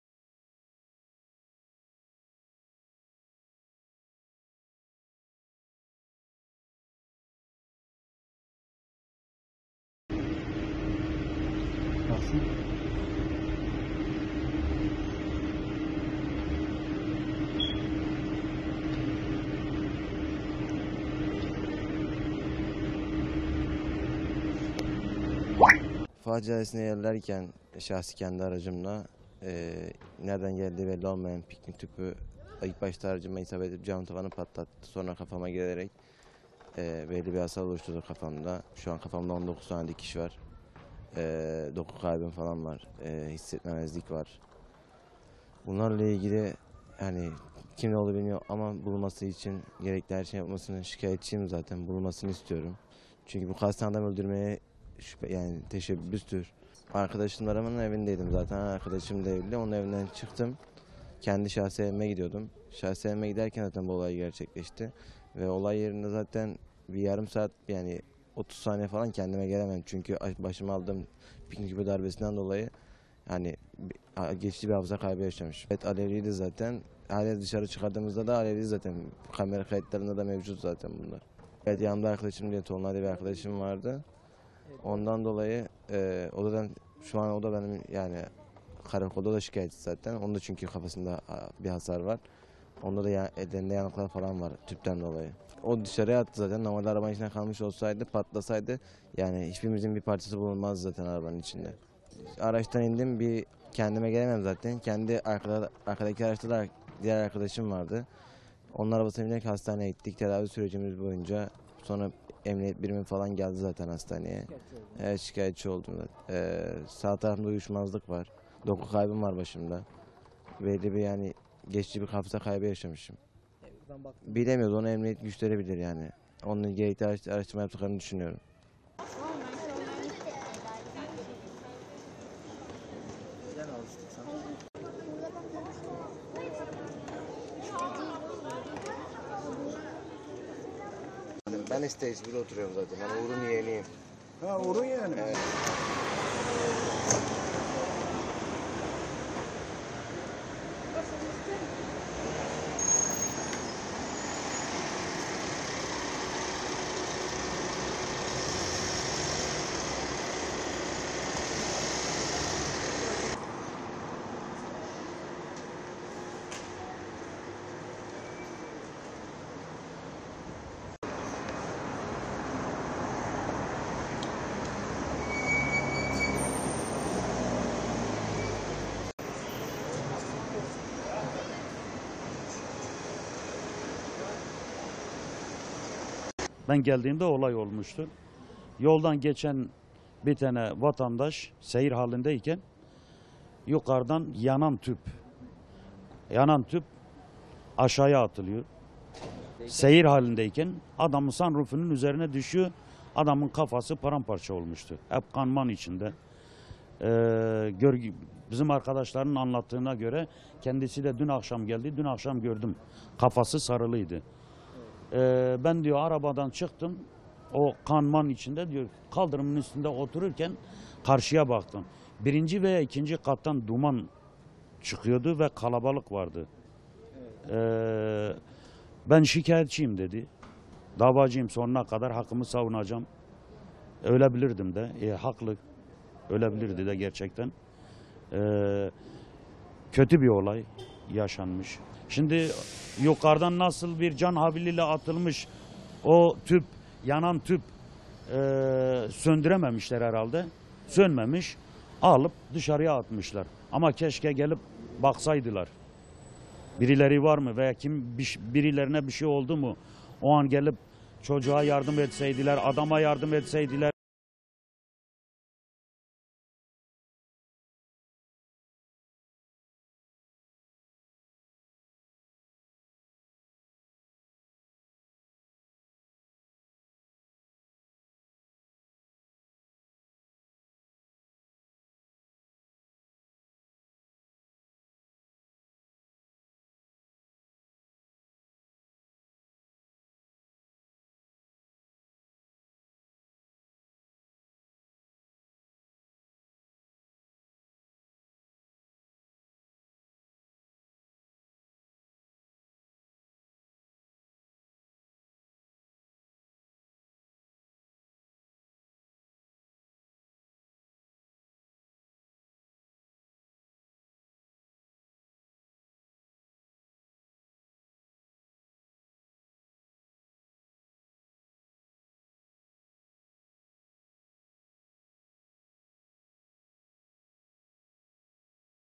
Bahçelievler'de Akılalmaz Olay Alev Alınca Balkondan Atılan Tüp Sürücünün Kafasına Düştü
BAHÇELİEVLER’de alev aldıktan sonra balkondan atılan tüp, o sırada seyir halinde olan otomobilin cam tavanını kırarak 24 yaşındaki sürücü Mehmet İşler’in kafasına isabet etti. Başından yaralanarak hastaneye kaldırılan Mehmet İşler’e 19 dikiş atılırken, olay anı güvenlik kamerasına yansıdı. Mehmet İşler, “Kim olduğunu bilmiyorum ama bulunması için gerekli her şeyin yapılmasını istiyorum. Çünkü bu kasten öldürmeye teşebbüstür” dedi.
Olay, Fevzi Çakmak Mahallesi’nde geçen Cumartesi günü meydana geldi. İddiaya göre, 24 yaşındaki Mehmet İşler, arkadaşıyla birlikte evine gitmek için Fatih Caddesi’nde otomobiliyle seyir halindeyken, alev alan tüp bir apartmanın balkonundan aşağı atıldı. Yanan tüp Mehmet İşler’in arabasının cam tavanına düştü. Camın kırılmasıyla yanan tüp, sürücü Mehmet İşler’i başına isabet etti. O esnada da otomobil içinde olan İşler’in arkadaşı ise yanan tüpü aracın dışına çıkararak patlama korkusuyla söndürmek için yol kenarına attı. Başından yaralanan Mehmet İşler, arkadaşları tarafından hastaneye götürüldü. Mehmet İşler’in kafasına hastanede 19 dikiş atıldı. İşler, hastanede bir gün kaldıktan sonra taburcu edildi. Talihsiz sürücü Mehmet İşler, yanan tüpü atan şüpheli veya şüphelilerin bulunması için karakola giderek şikayetçi oldu. İşler’in otomobilinde ise hasar oluştu.
“TÜPÜN DARBESİNDEN DOLAYI GEÇİCİ BİR HAFIZA KAYBI YAŞAMIŞIM”
Tüpün başına isabet etmesi sonucu yaralanan Mehmet İşler, “Caddede ilerlerken kendi aracımla, nereden geldiği belli olmayan piknik tüpü ilk başta aracıma isabet edip cam tavanı patlattı. Sonra kafama girerek belli bir hasar oluşturdu kafamda. Şu an kafamda 19 dikiş var. Doku kaybım falan var, hissetmemezlik var. Bununla ilgili kim olduğunu bilmiyorum ama bulunması için gerekli her şeyin yapılmasını istiyorum. Şikayetçiyim zaten bulunmasını istiyorum. Çünkü bu kasten öldürmeye teşebbüstür” dedi.
O gün yaşadıklarını anlatan İşler, “Arkadaşımın evindeydim, onun evinden çıktım. Kendi evime gidiyordum. Evime giderken zaten bu olay gerçekleşti. Ve olay yerinde zaten bir süre kendime gelemedim. Çünkü başıma aldığım piknik tüpünün darbesinden dolayı geçici bir hafıza kaybı yaşamışım. Alevliydi zaten, dışarı çıkardığımızda da alevi zaten kamera kayıtlarında da mevcut. Yanımda Tolunay diye arkadaşım vardı. O da karakolda şikayetçi oldu. Onun da çünkü kafasında bir hasar var, ellerinde yanıklar falan var tüpten dolayı. O dışarıya attı zaten. Normalde arabanın içinde kalmış olsaydı, patlasaydı yani hiçbirimizin parçası bulunmazdı zaten arabanın içinde. Araçtan indim bir süre kendime gelemedim zaten. Arkadaki araçta da diğer arkadaşım vardı. Onun arabasıyla direk hastaneye gittik. Tedavi süreci sonrasında emniyet birimleri geldi zaten hastaneye. Şikayetçi oldum. Sağ tarafımda uyuşmazlık var. Doku kaybım var başımda. Yani geçici bir hafıza kaybı yaşadım” şeklinde konuştu.
“YUKARIDAN YANAN TÜP AŞAĞIYA ATILIYOR”
Cadde üzerinde işletme sahibi Aydın Karataş, “Ben geldiğimde olay olmuştu. Yoldan geçen bir vatandaş seyir halindeyken yukarıdan yanan tüp aşağıya atılıyor. Seyir halindeyken adamın sunroof’unun üzerine düşüyor. Ölebilirdi de gerçekten. Kötü bir olay yaşanmış. Şimdi yukarıdan nasıl bir can havliyle atılmış o tüp; yanan tüpü, söndürememişler herhalde. Sönmemiş, alıp dışarıya atmışlar. Ama keşke gelip baksaydılar birileri var mı veya birilerine bir şey oldu mu? O an gelip çocuğa yardım etseydiler, adama yardım etseydiler” diye konuştu. Polisin şüpheliyi tespit etmek için çalışmasının sürdüğü öğrenildi.
Öte yandan yaşanan olay güvenlik kamerasına yansıdı. Görüntülerde İşler’e ait otomobil cadde üzerinde seyir halindeyken apartmanın balkonundan yanan tüp otomobilin cam tavanına düşüyor. Otomobilin içinden alevli şekilde çıkarılan tüp İşler’in arkadaşları tarafından uzaklaştırılmaya çalışılıyor.